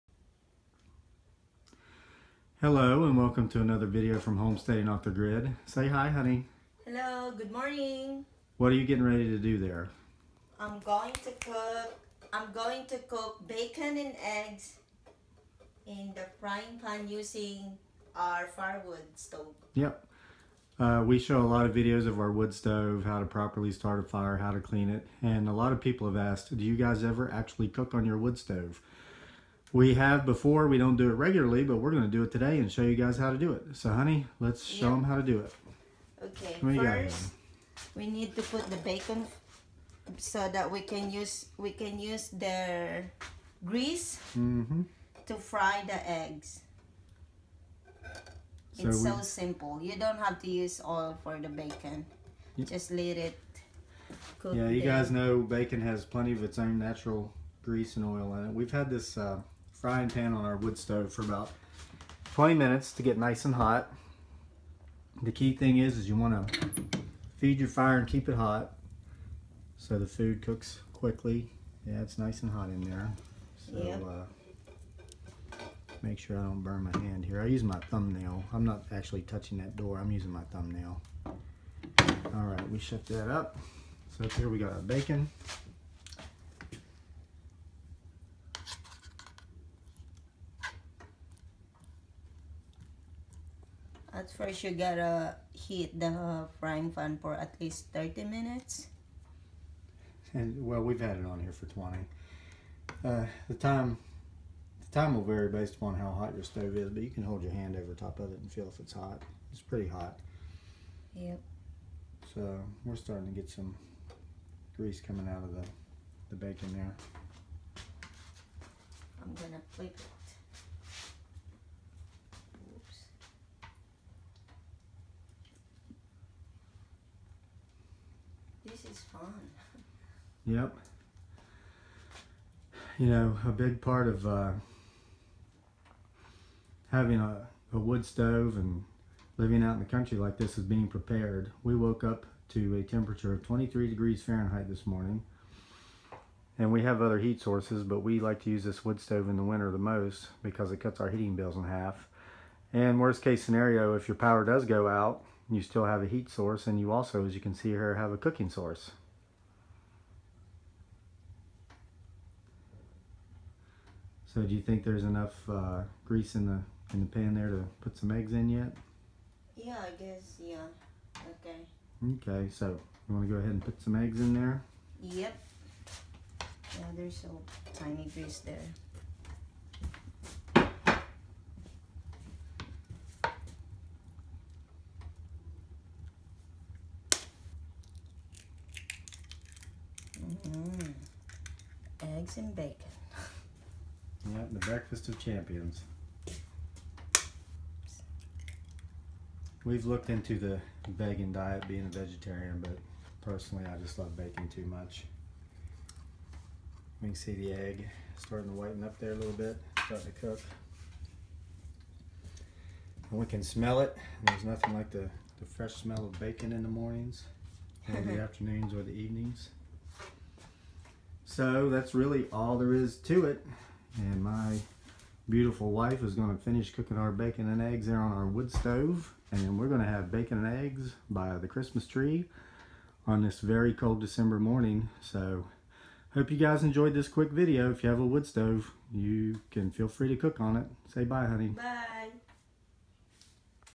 Cooking On A Cast Iron Wood Stove
We decided to cook our breakfast, bacon and eggs, on our cast iron wood stove this morning and we thought we'd share it with you.